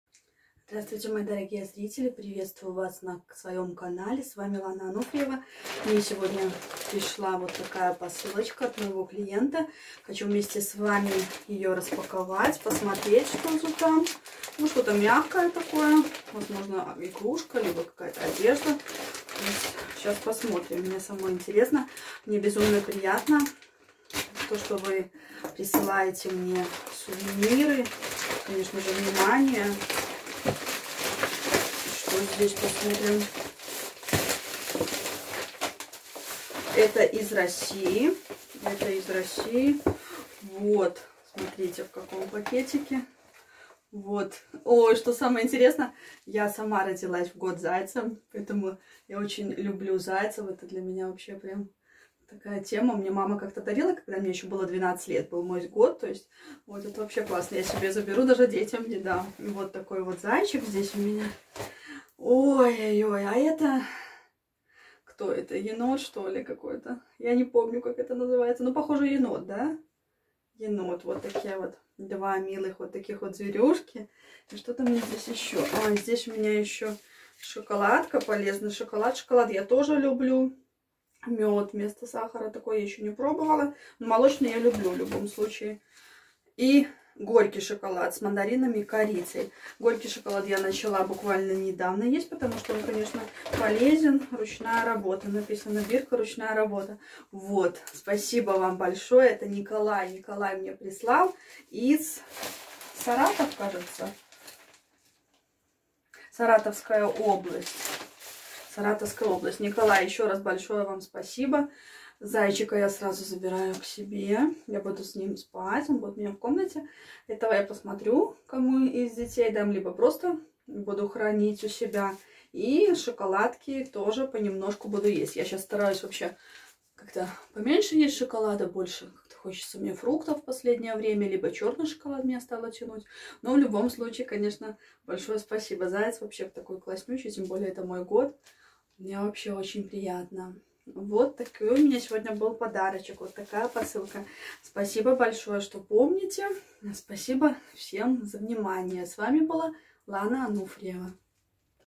Подарок 🎁 от Николая
Подарок от Николая. Подарок своими руками.  Милые игрушки в подарок. Распаковка подарка. Подарок от благодарного клиента
Все любят подарки, и я не исключение)
Спасибо за Ваши подарки!!!